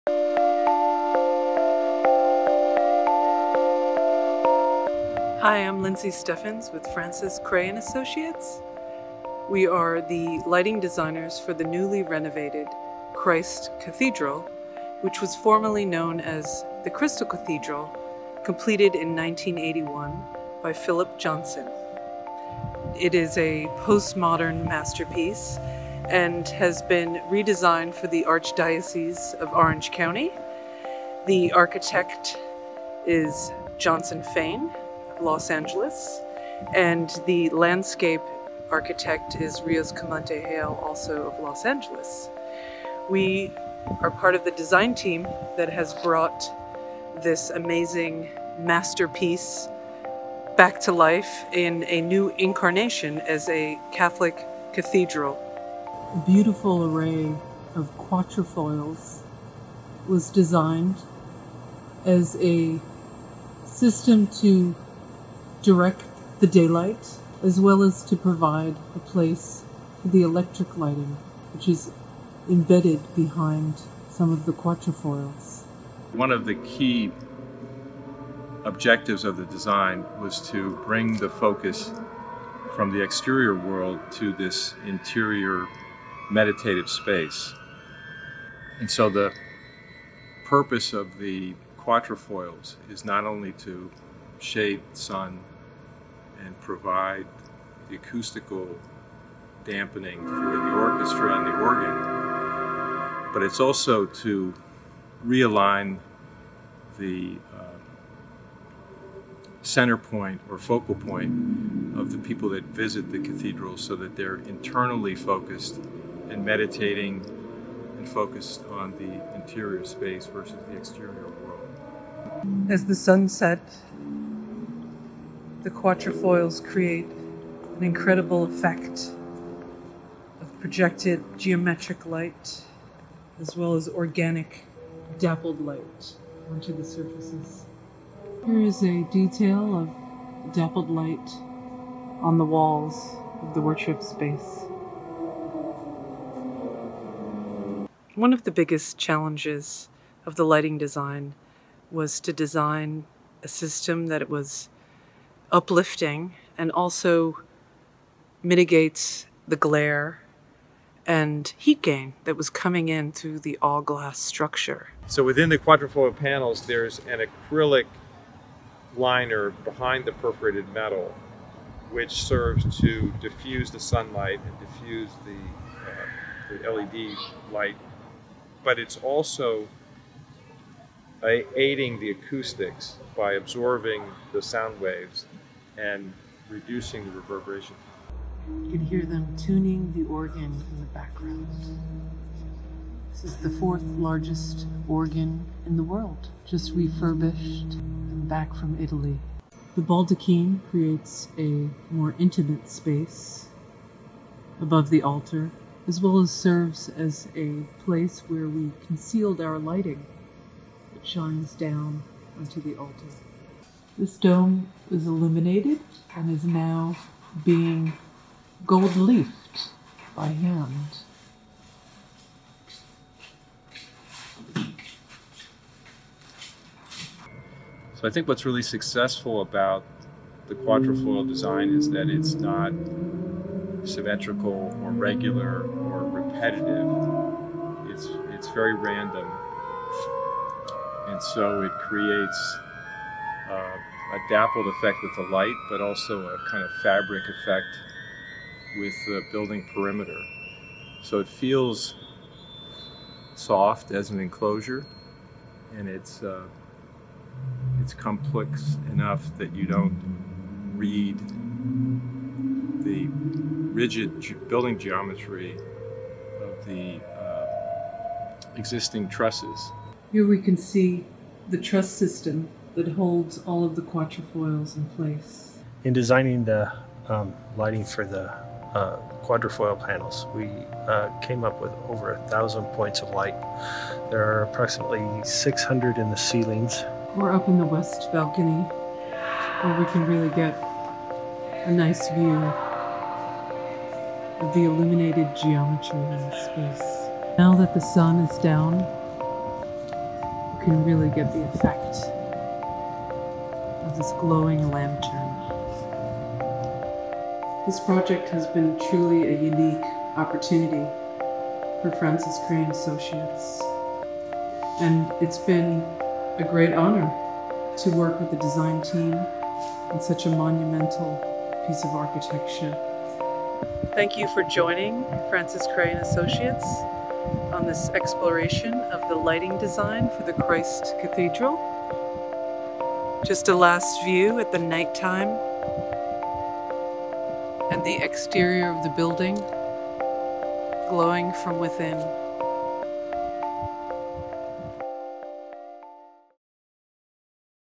Christ Cathedral AGdoc cut
A postmodern masterpiece, the Crystal Cathedral, was designed by world-renowned architect Philip Johnson and his partner John Burgee  and completed in 1981. For the re-design, the Crystal Cathedral became Christ Cathedral of the Diocese of Orange County in southern California. The famous landmark is designed in a four-pointed star pattern and features more than 10,000 panes of glass. The design team for the mass renovation was Johnson Fain (architecture), Rios Clementi Hale (Landscape architecture) and Francis Krahe and Associates (lighting design).

"The redesign of the surfaces addresses acoustics, daylight and night lighting, solar heat transmission and ventilation to ensure optimal performance and comfort of visitors. In order to regulate both lighting and comfort, the firm has designed an algorithmically complex series of quatrefoils made up of triangular metal sails. Arranged in various states of openness on the inside surface of the glass frames, the sails will modulate light, reduce glare and cast beautiful patterns on the cathedral interior."-Diocese of Orange